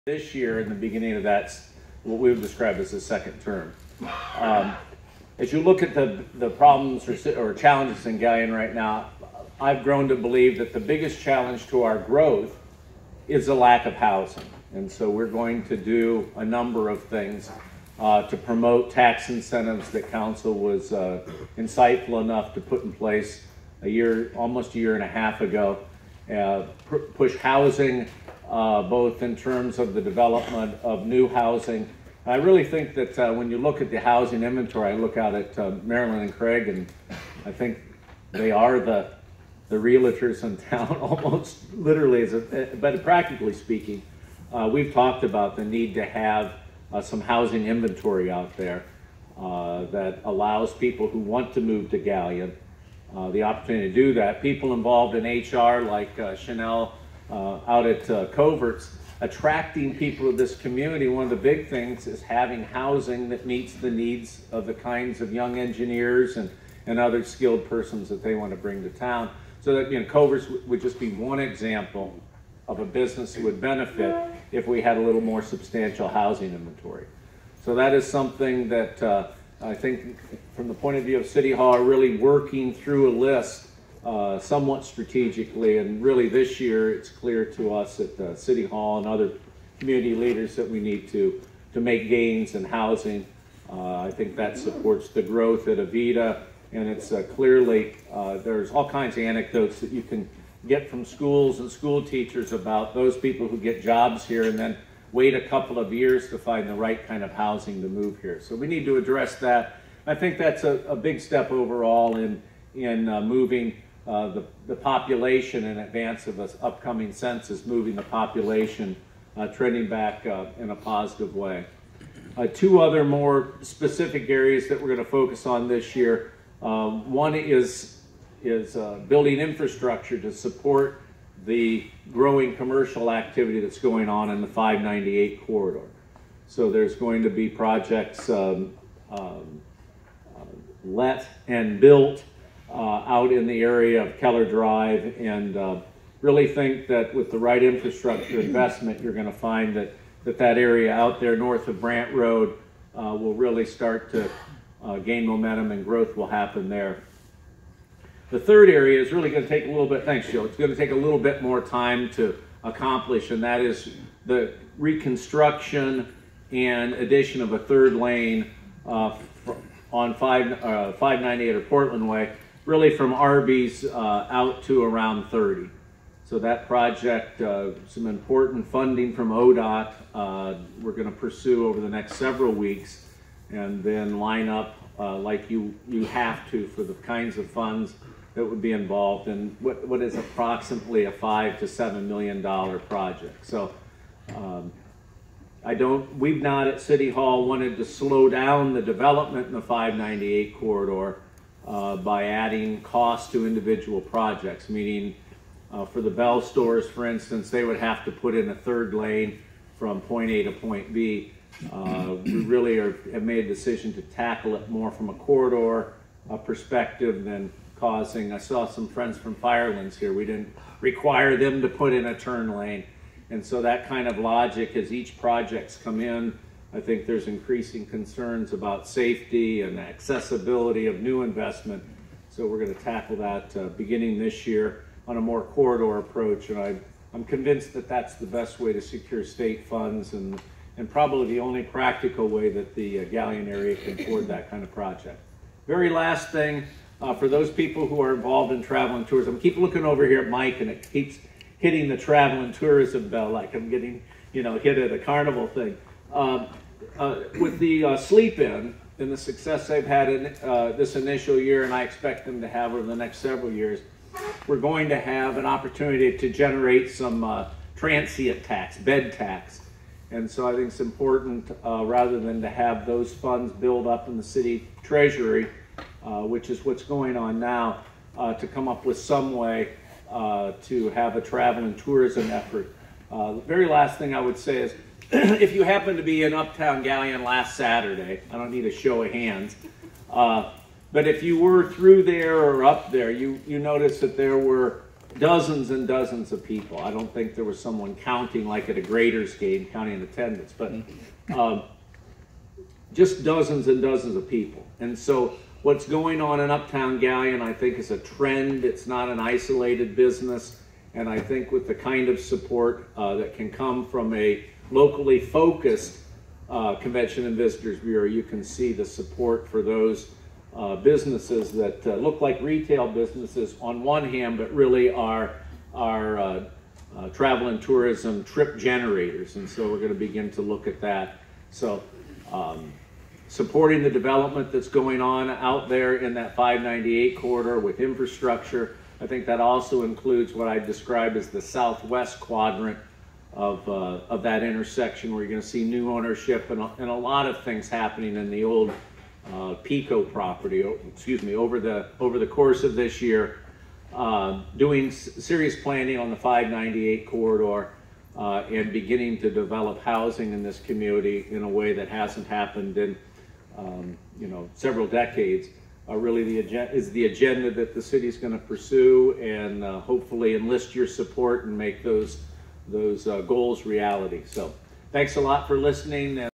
Chamber Breakfast 2018
Mayor Tom O'Leary speaks during the Chamber of Commerce Forecast Breakfast on March 6